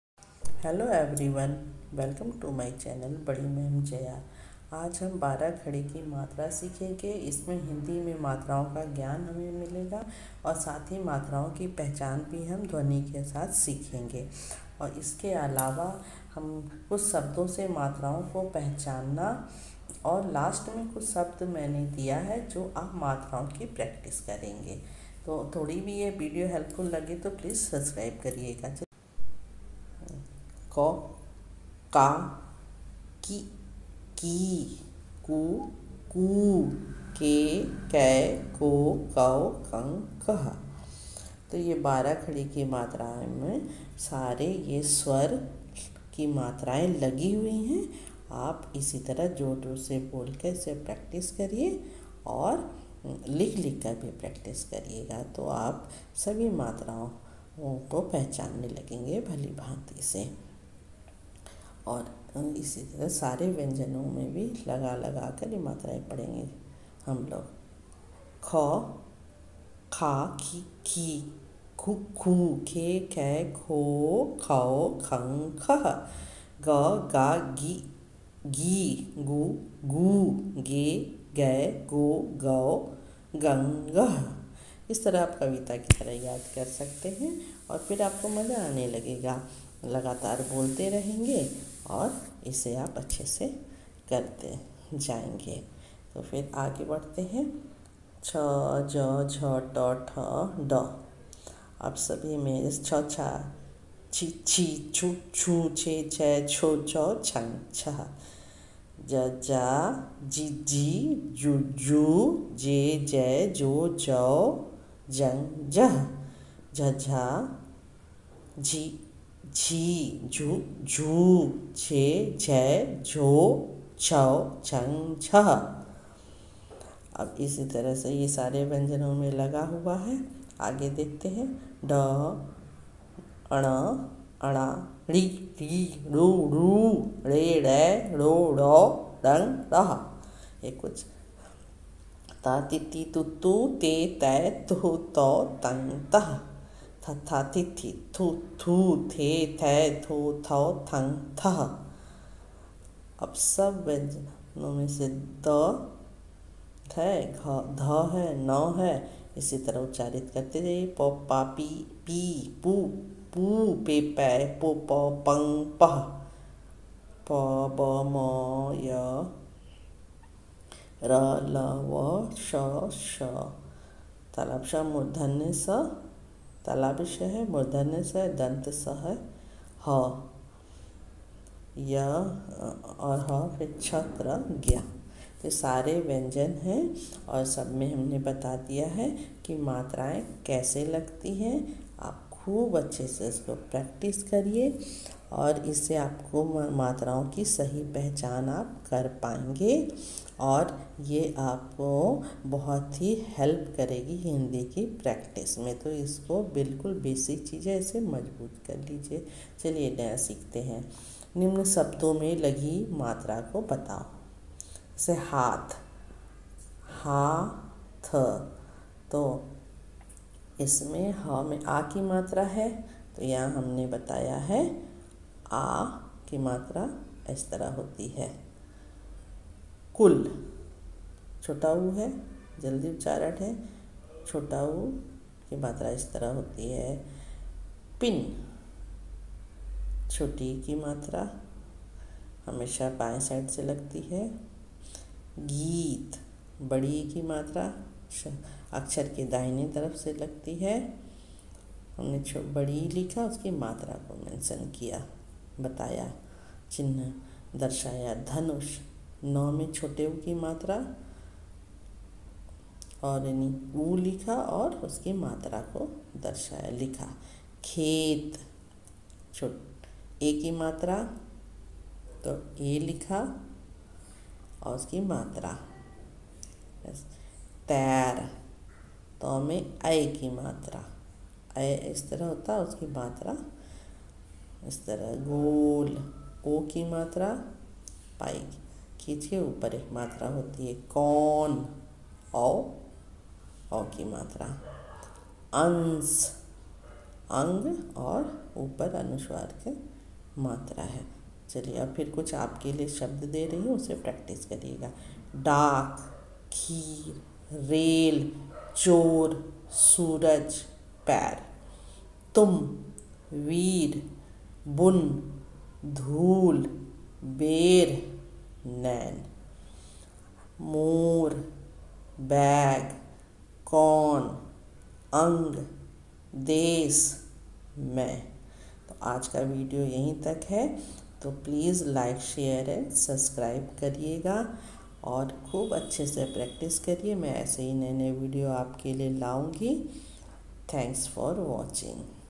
हिन्दी मात्रा ज्ञान |मात्रा कैसे सीखे |बारह खड़ी की मात्रा | Hindi matra learning with Example l
आज की विडियो में बारह खड़ी की मात्रा पढना सीखेंगे और मात्रा ओ का ज्ञान एवं पहचान ध्वनि के साथ सीखेंगे